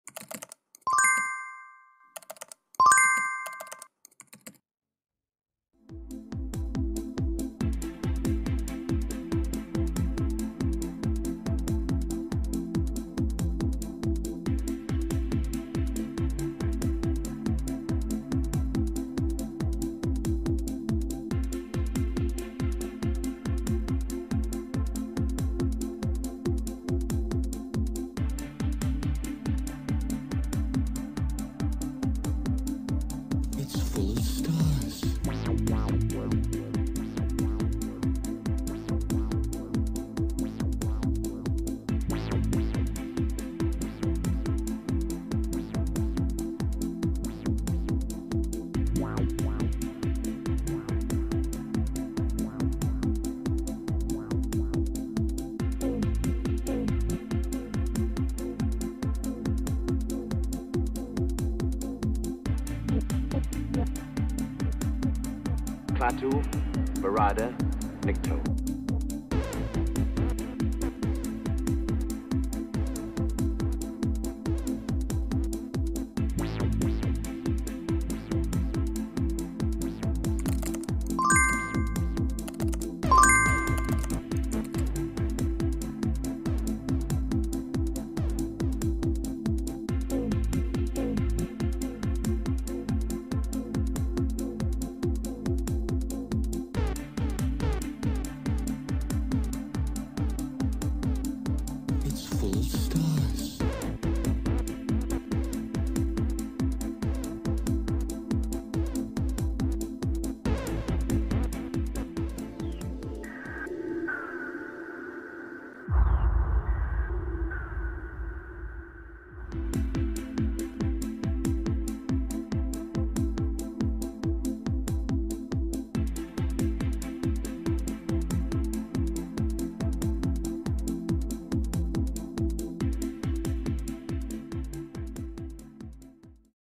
Ovni Nodriza apareció en Reynosa, Tamaulipas y asi la grabaron
Testigos grabaron este 25 de Marzo 2025 en Reynosa Tamaulipas, una nave nodriza que apareció a gran altura emitiendo luces y cambiando su forma.

Comentaron finalmente que rápidamente se elevó hasta desaparecer